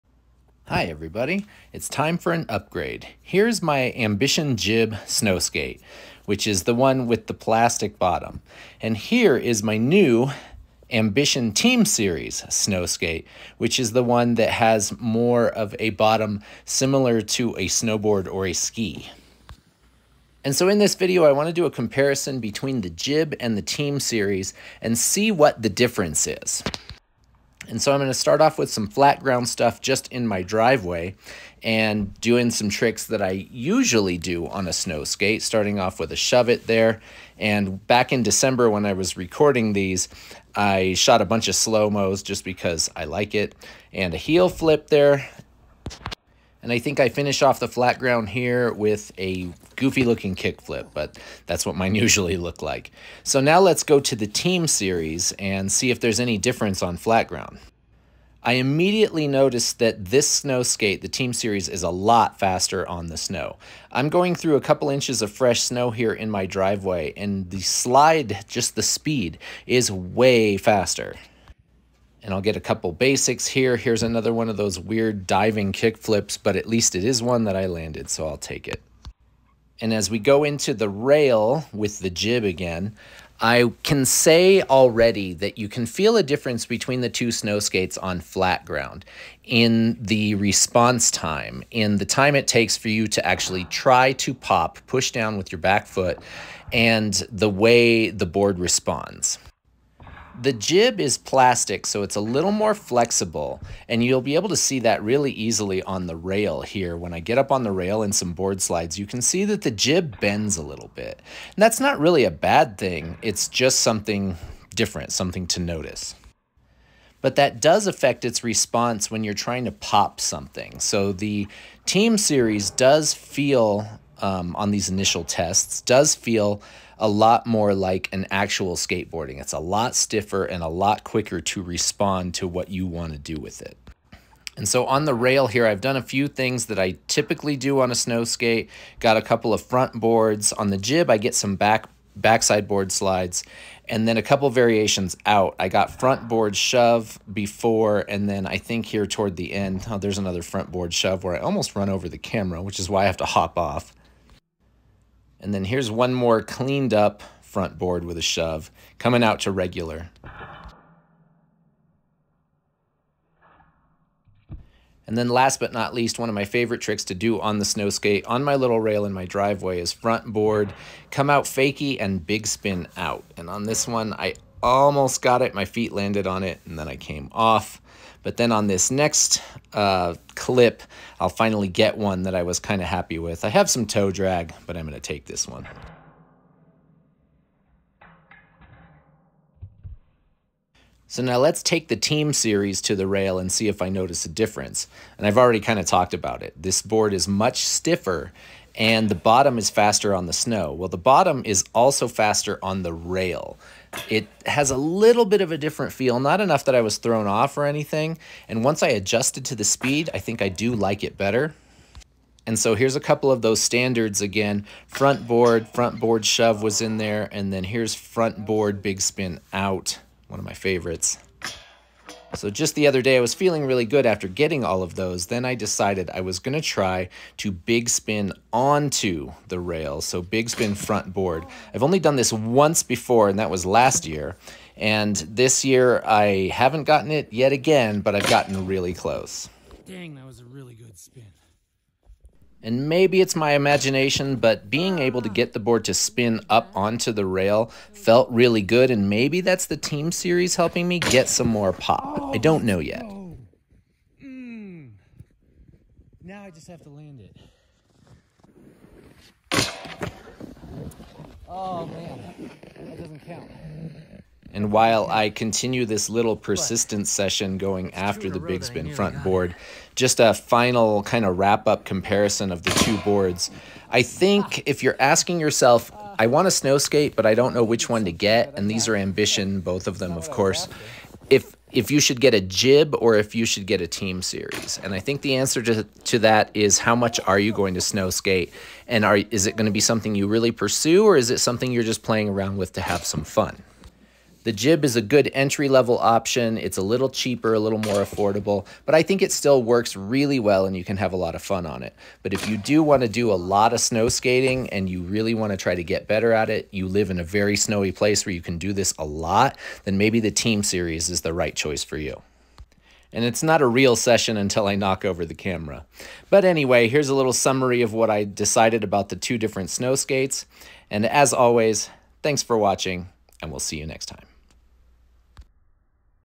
Comparing the Ambition Jib and Team Series Snowskates
Are you thinking about taking up snowskating? I’m here to help! Today, I’m comparing my Ambition Jib vs my new Ambition Team Series. What’s the difference and which one should I buy? Watch to the end to find out.